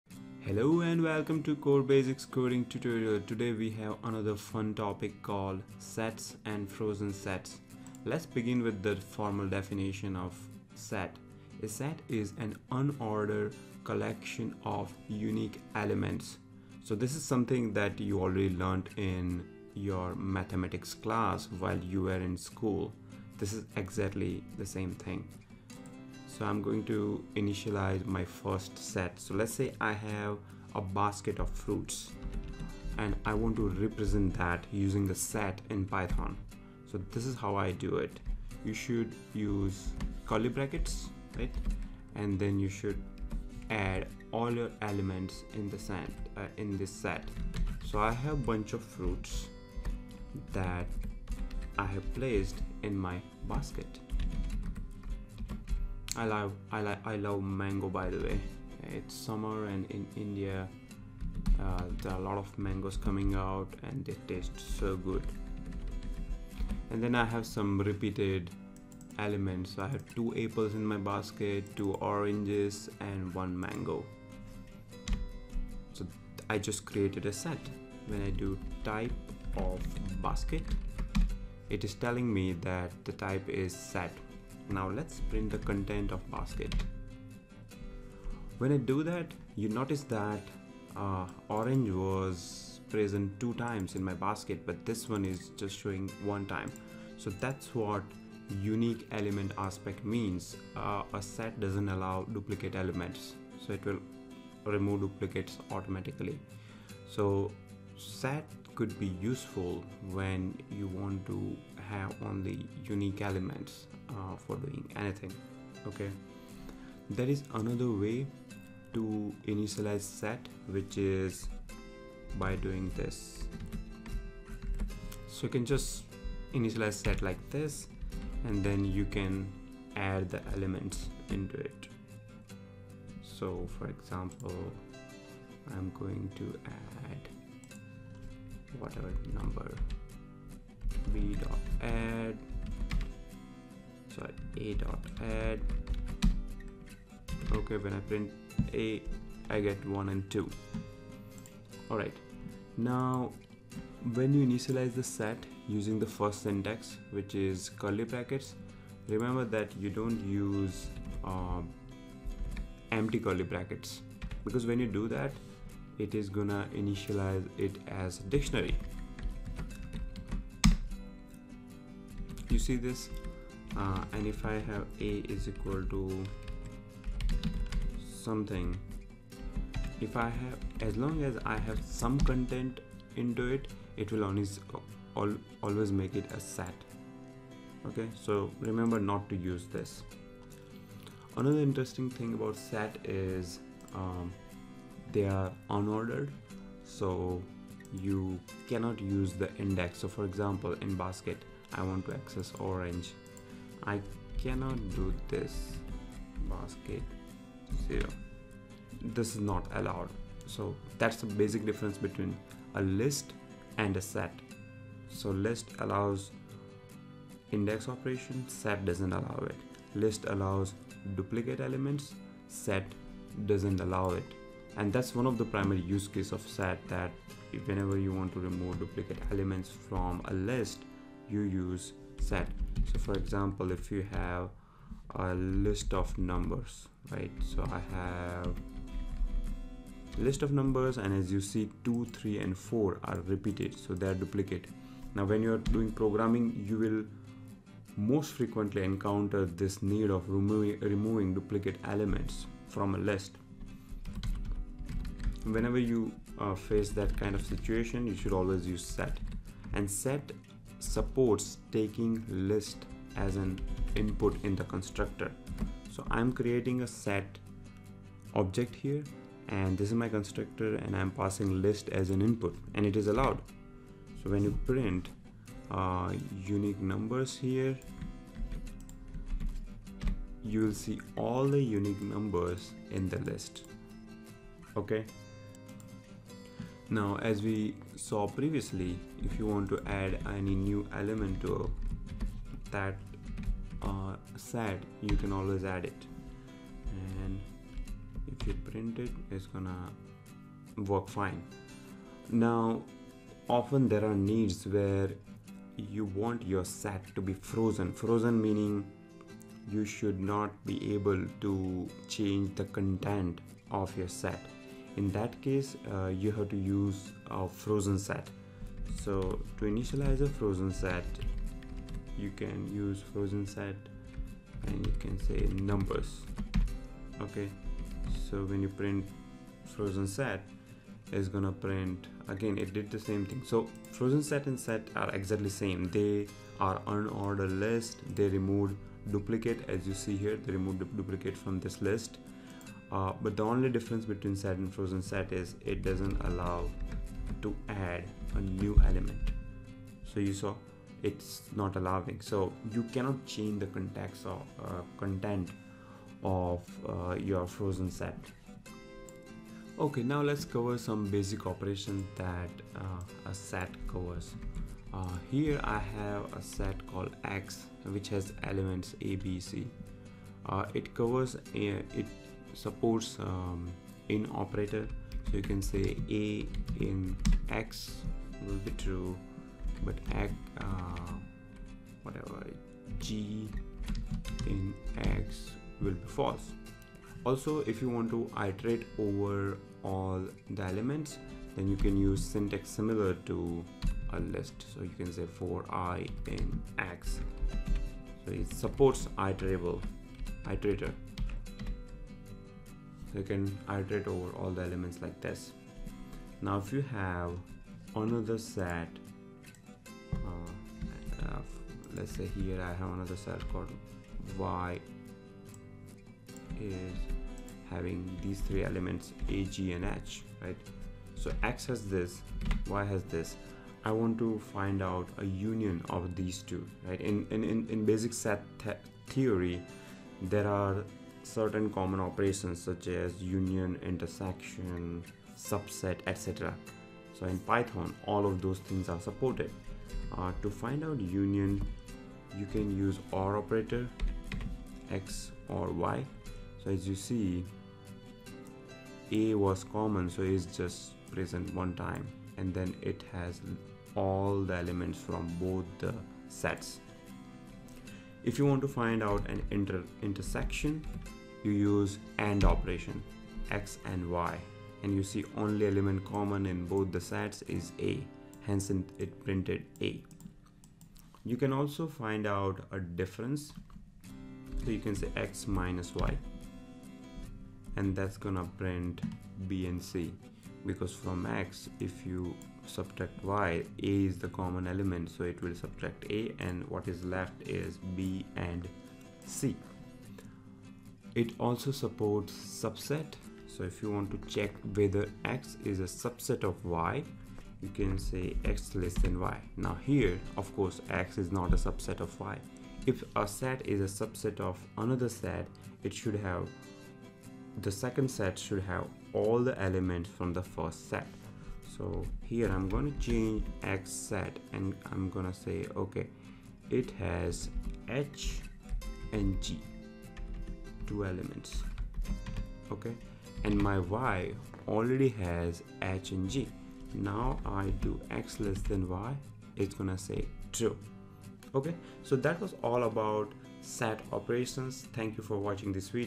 Python Tutorial - 24. Sets and Frozen Sets
This tutorial will explain about “sets and frozen sets”. The major topics highlighted in this video are what is set, examples of set, operations of set, what is “frozen set”, the difference between “list and set”, what is “in operator”, what is union, intersect, suprect and subset operation of set.

Topics that are covered in this Python Video:
0:00 Introduction
0:08 Definition of Set and example of set
3:11 Set is an Unordered
3:28 List vs set basics difference
5:04 What is frozen set and its example
5:59 implement basics operation of set
6:12 "in" operator
7:06 Union, intersect, suprect and subset operation of set

Learn how to use sets and frozen sets in python. Set is basically mathematical set and could be really useful in programming. Frozen sets are same as set except you can not change them (immutable).